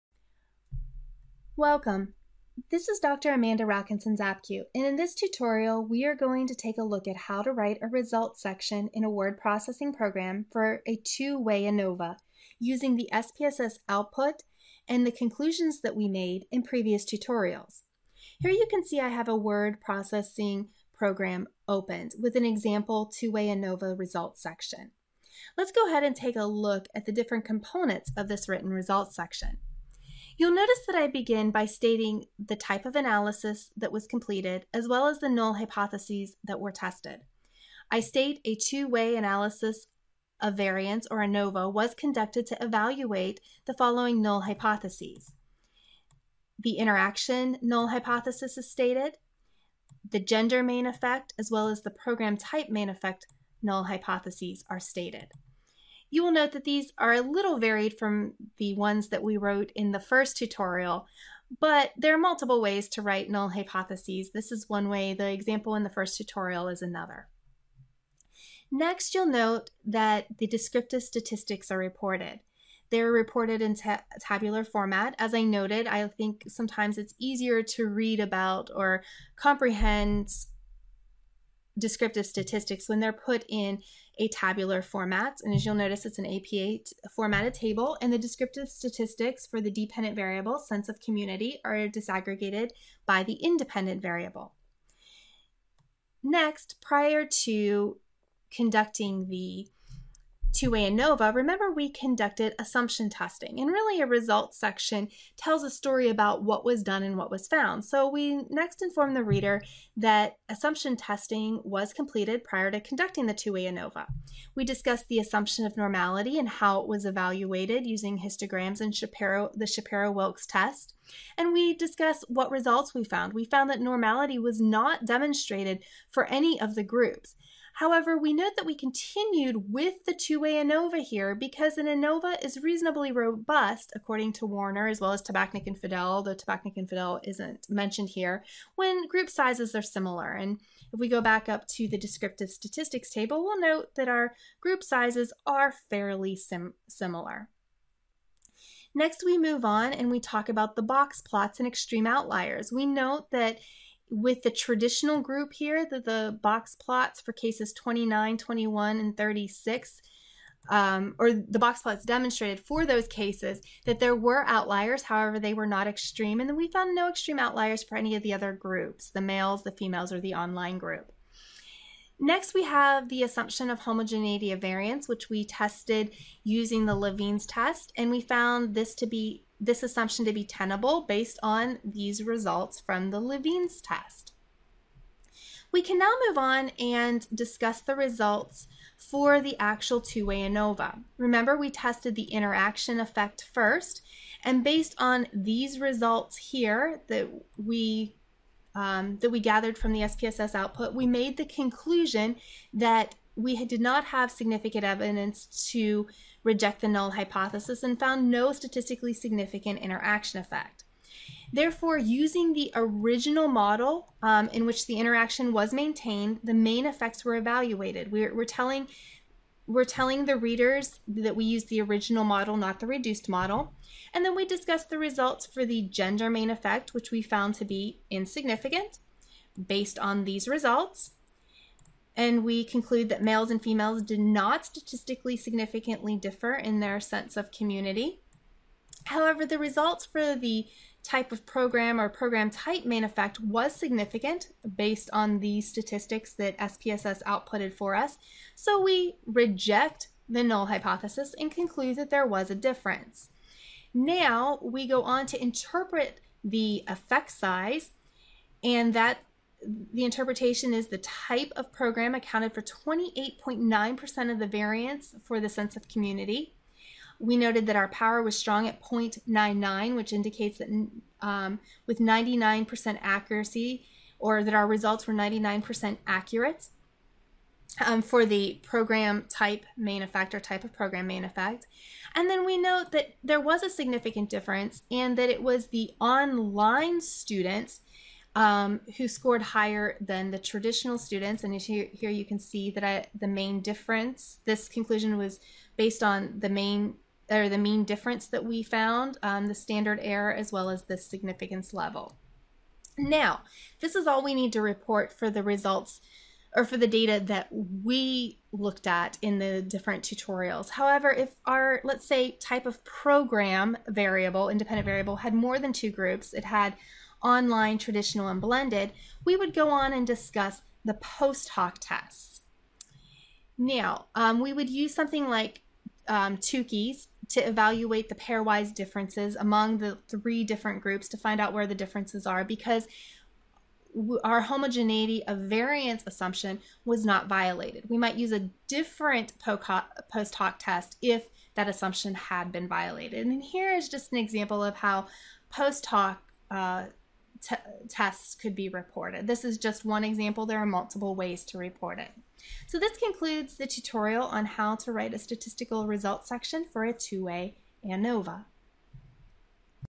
The Doctoral Journey: Writing the Results Section for a Two-Way ANOVA [Part 10 of 10]
In this doctoral journey 10-part video, Dr. Rockinson-Szapkiw shows you how to conduct a two-way AVOVA using SPSS.